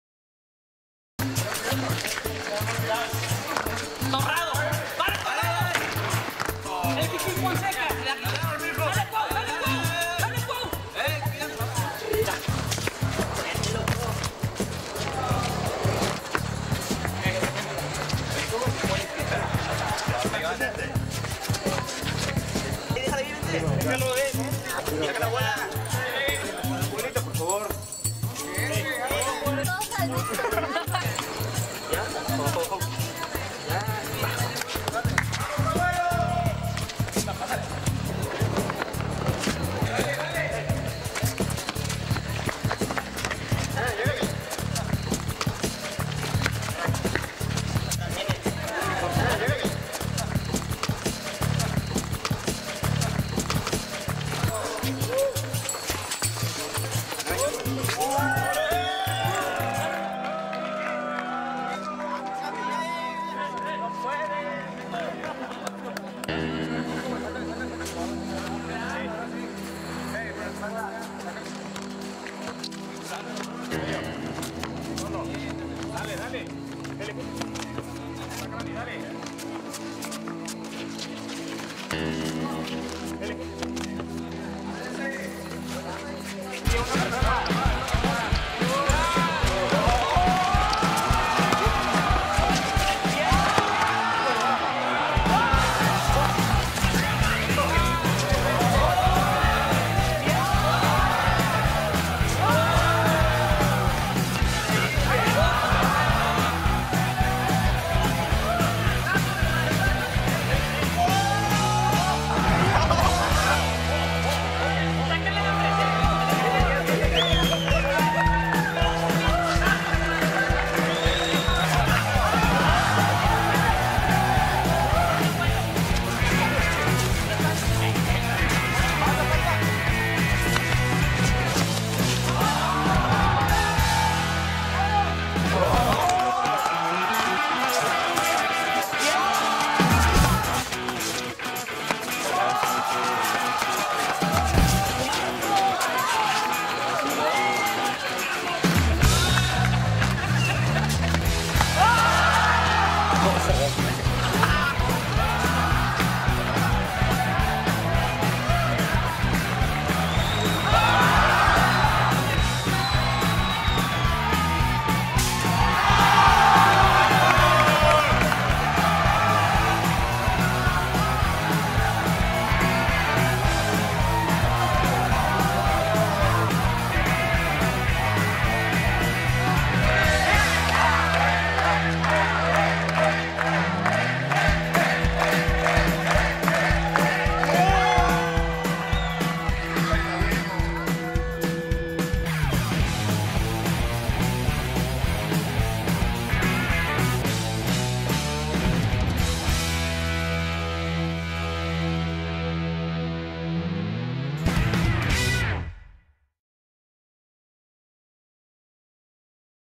足球神人喬裝成老頭子跑到球場
花式足球明星賈尼爾(Séan Garnier)